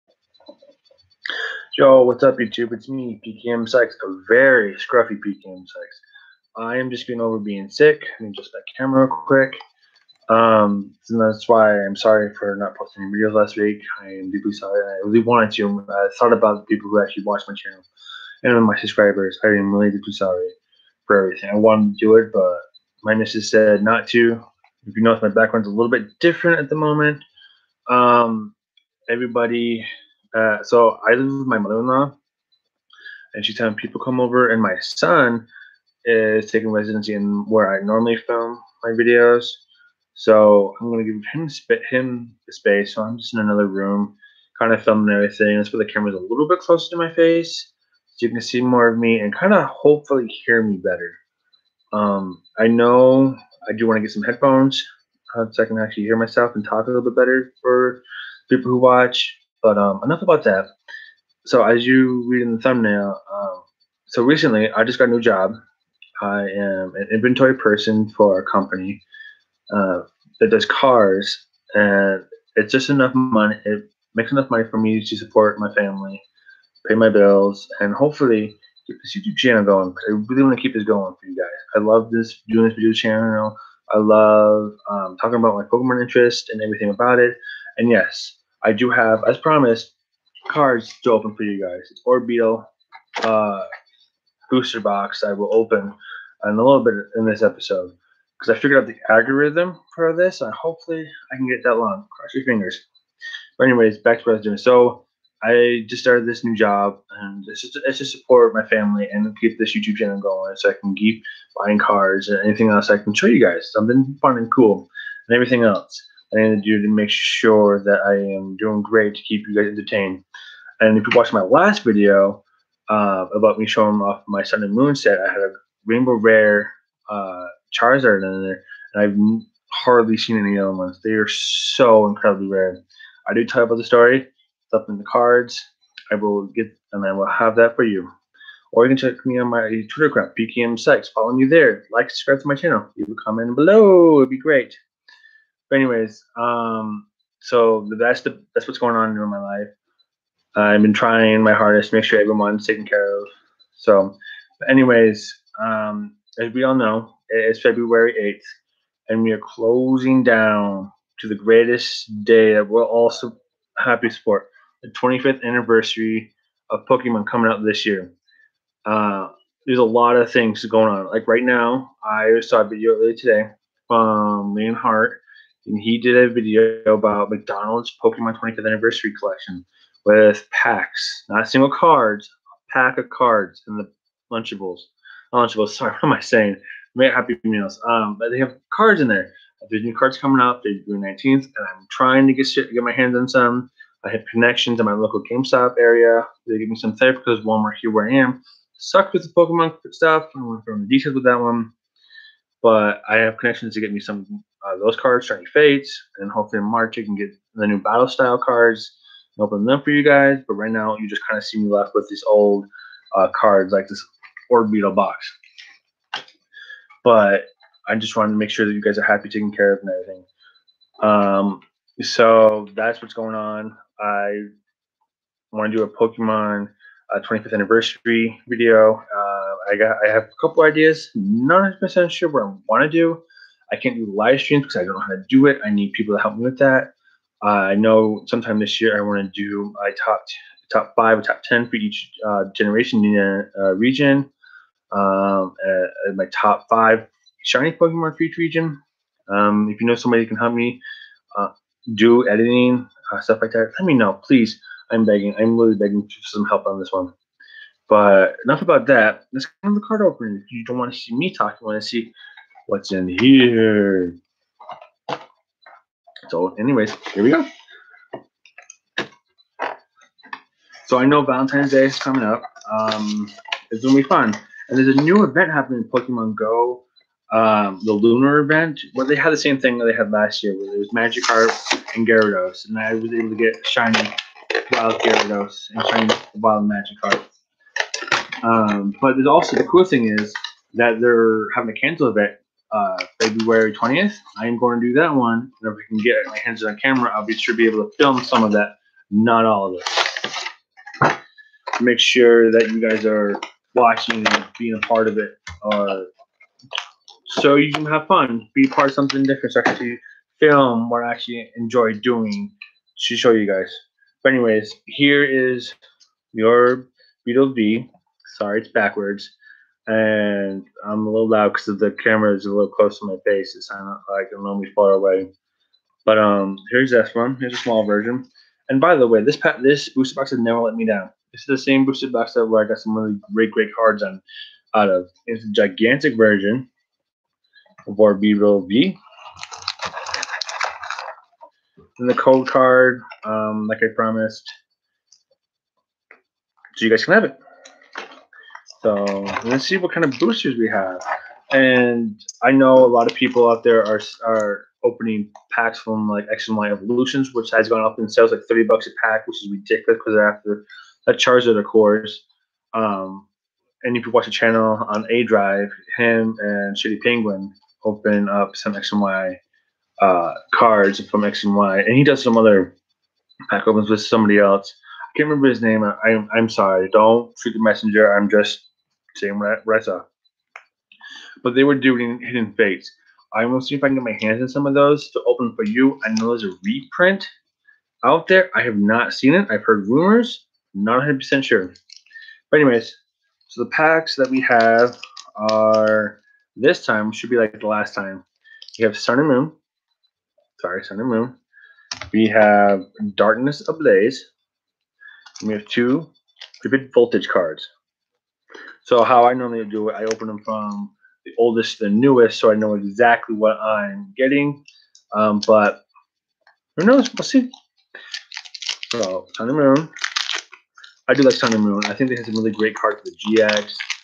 Talking about what's new in my life, Money wise and Pokémon card opening
Talking about what`s new in my life with my new job, and trying to get more Pokémon fun going on this channel, since I that is what I want to do. Plus kind of upset that the video was cut short to my Wifi signal quite on me during the video, so no  pack openings and didn`t released it till t was to late